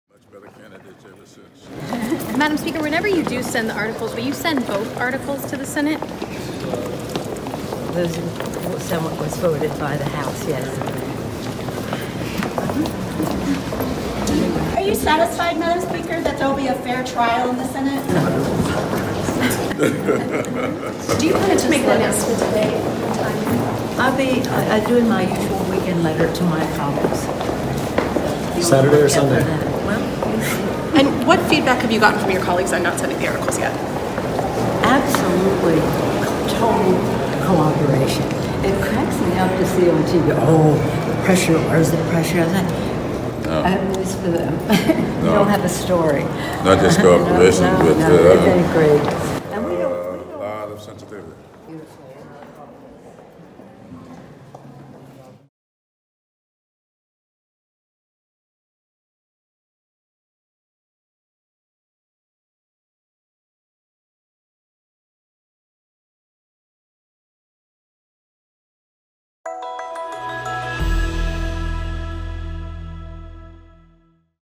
[글로벌 나우] 펠로시 의장, 탄핵안 상원 제출 예고
낸시 펠로시 미국 하원의장은 민주당 의원들에게 보낸 서한에서 도널드 트럼프 대통령 탄핵 결의안의 이번 주 상원 제출을 예고했다.